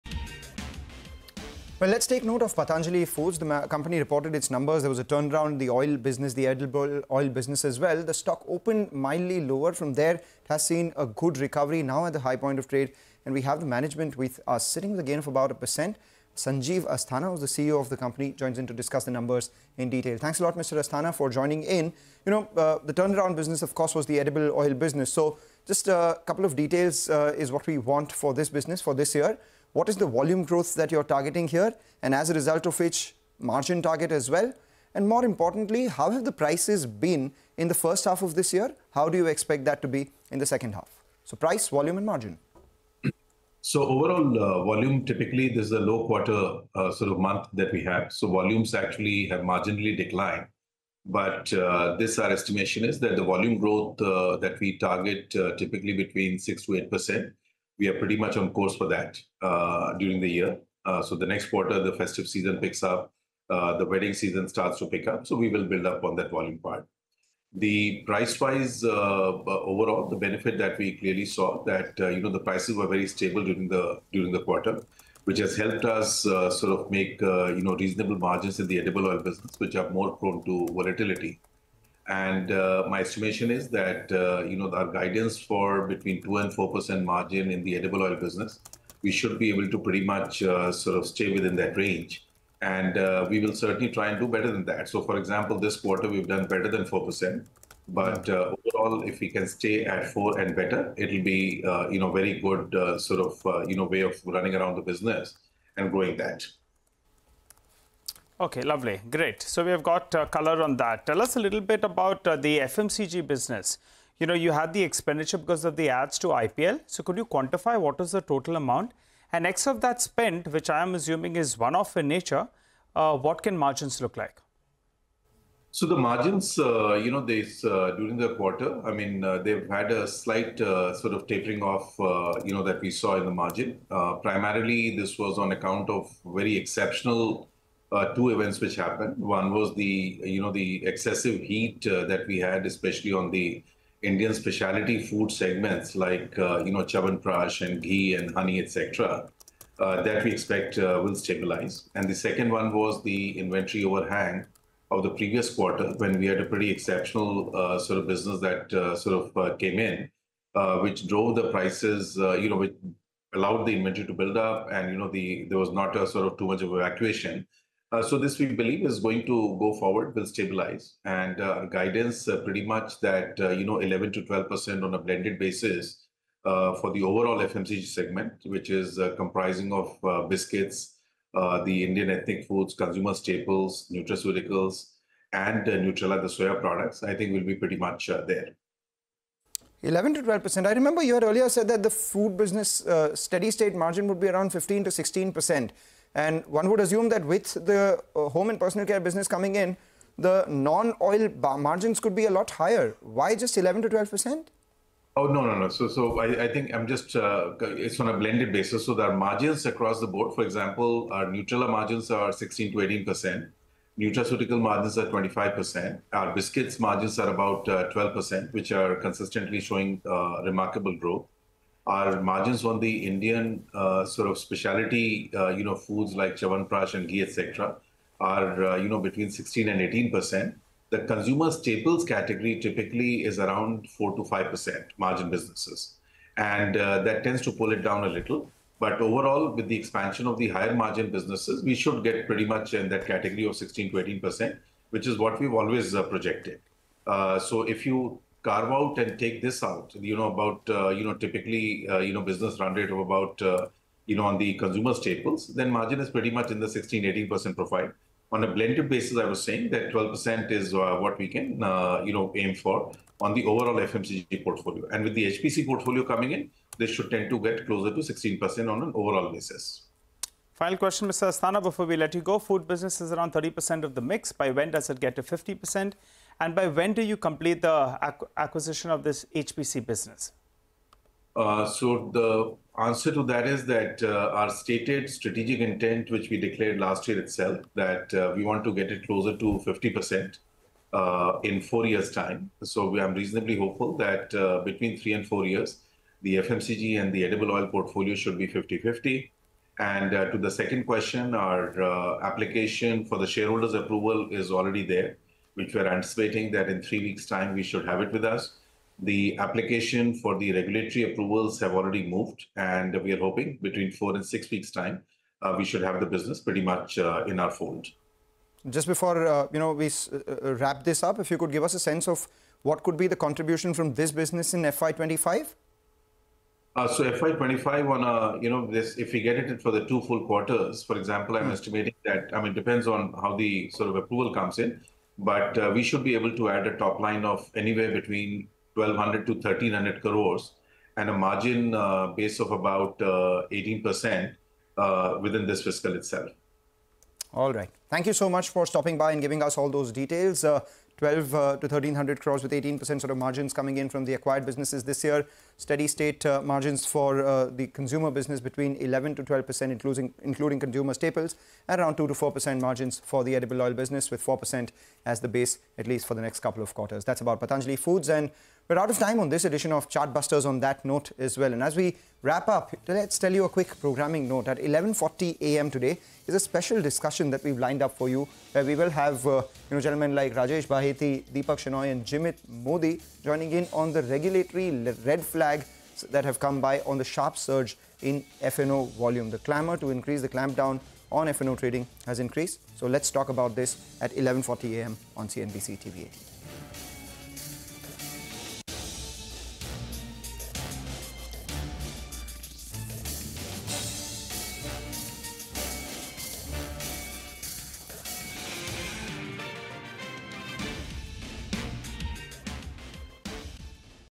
Volume Marginally Declined YoY And Margin Tapered Off In Q1FY25: Patanjali Foods | CNBC TV18
1QWithCNBCTV18 | Volume marginally declined YoY and margin tapered off in Q1FY25 says Sanjeev Asthana, CEO at Patanjali foods. Tells Mangalam Maloo & Nigel D'Souza that the company is on course for 6-8% volume growth in FY25.

n18oc_business